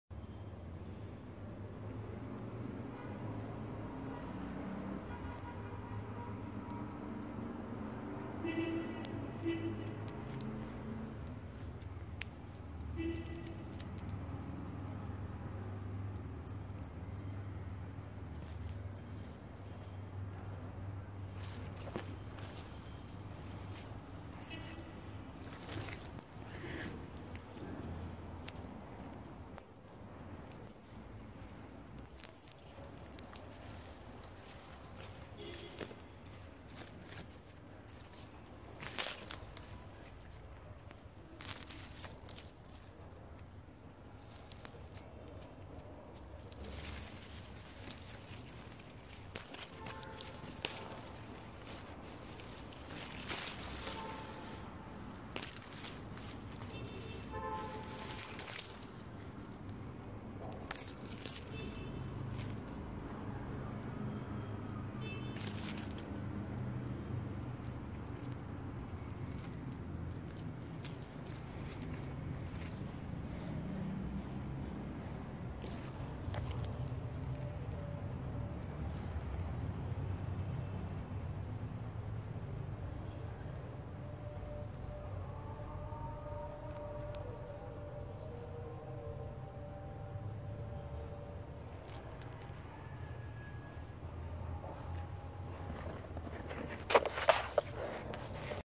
محصول الشطة اليوم ...جيت اقطف الشطة قطعت الفرع كله 😂😂
الزراعة المنزلية ...ما شاء الله شجرة الشطة ..انتاج يكفيكى طول الموسم والحمد لله